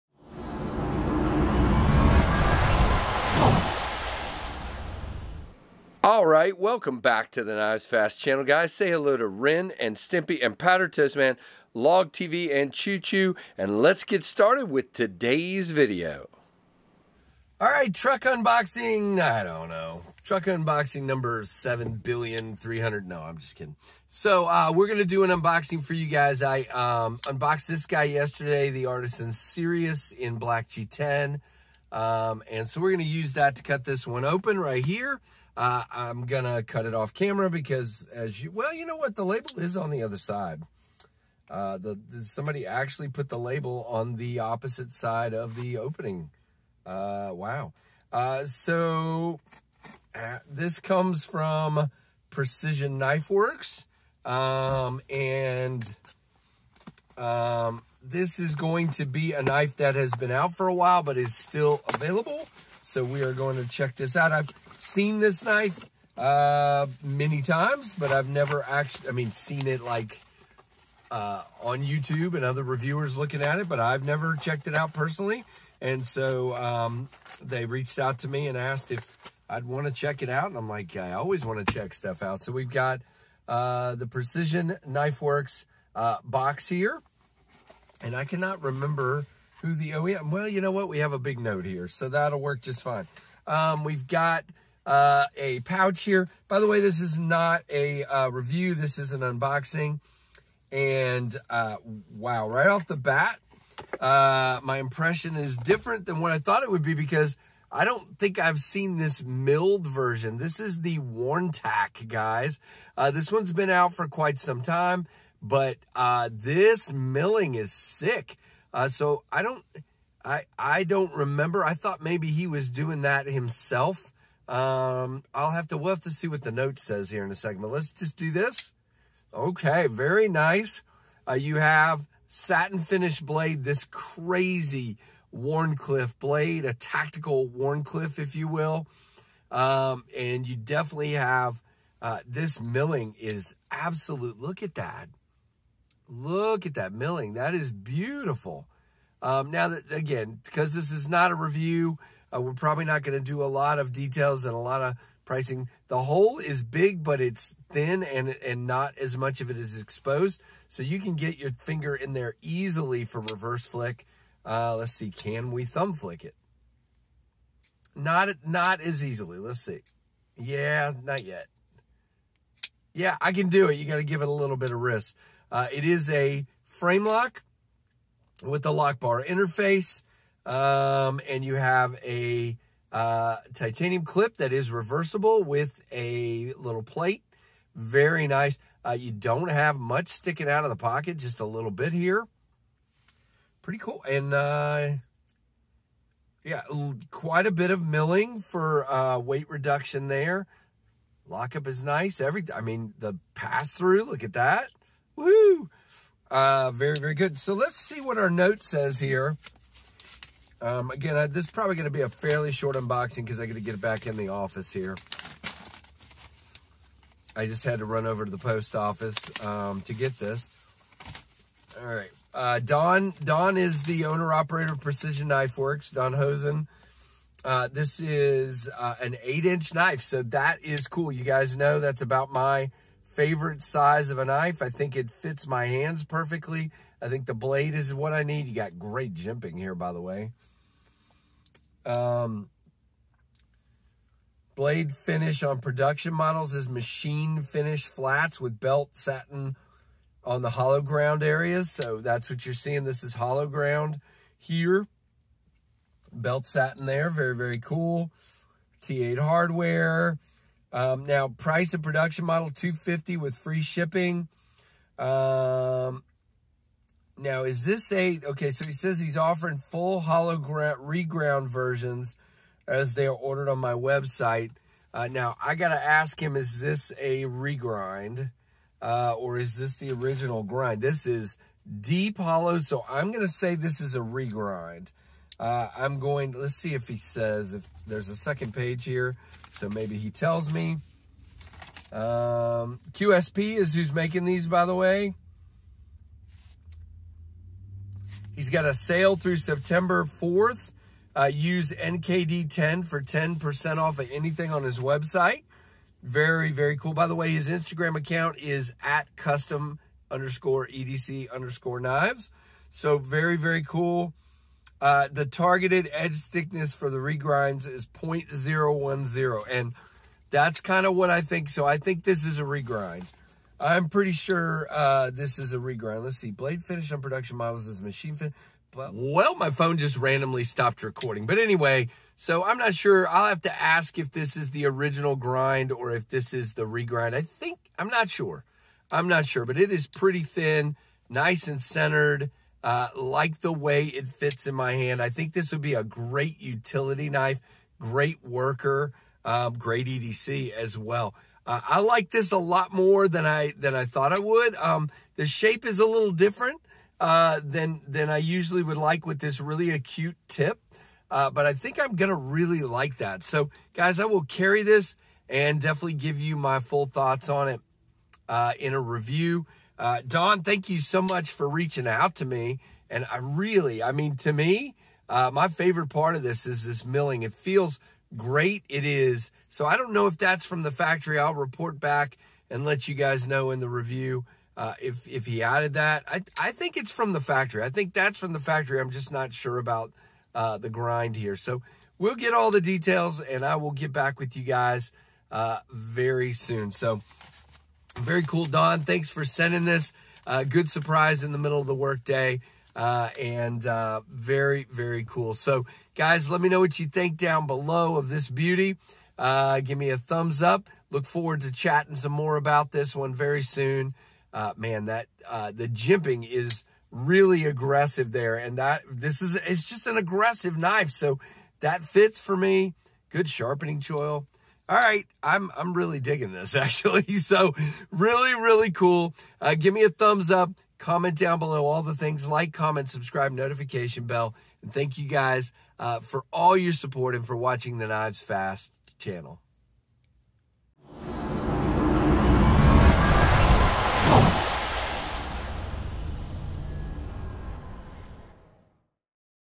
From the Truck: Unboxing a Precision Work Knife.
This is my truck unboxing of the Precision Knife Works Wharnetac.